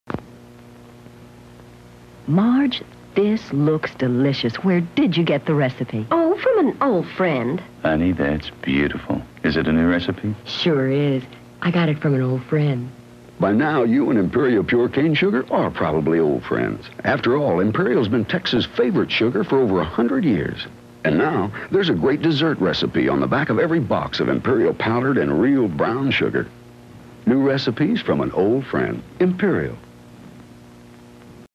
1977 Imperial Sugar New Recipes from an Old Friend TV Commercial
to get the Harvest Pecan Cake recipe featured in this commercial.